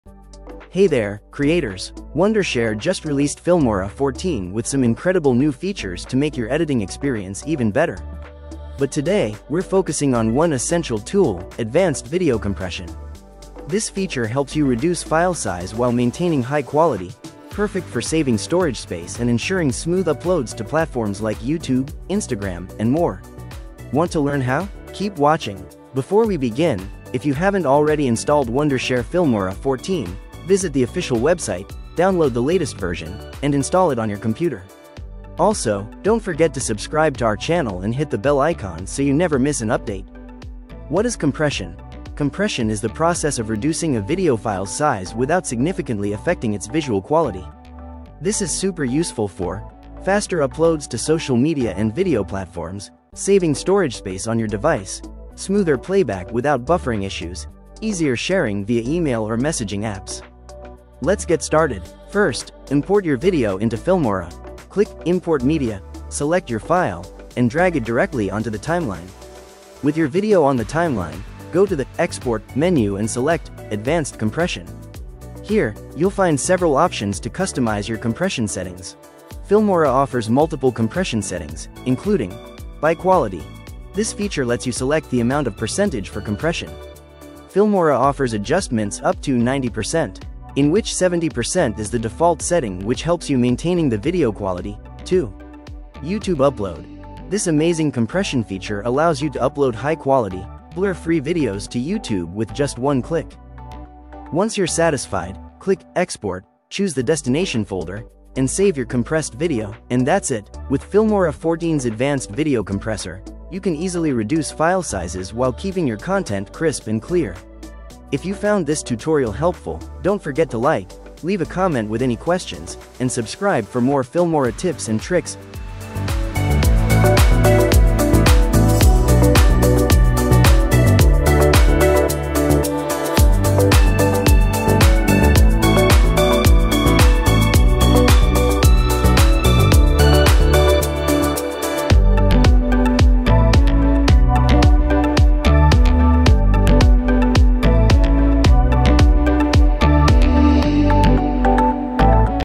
How To Compress Video Without Losing Quality in Filmora 14
Hey Guys, Abhishek here In this Filmora tutorial video,  I am going to show you, how you can compress video without losing quality in Filmora 14. Watch and learn Filmora in easy steps.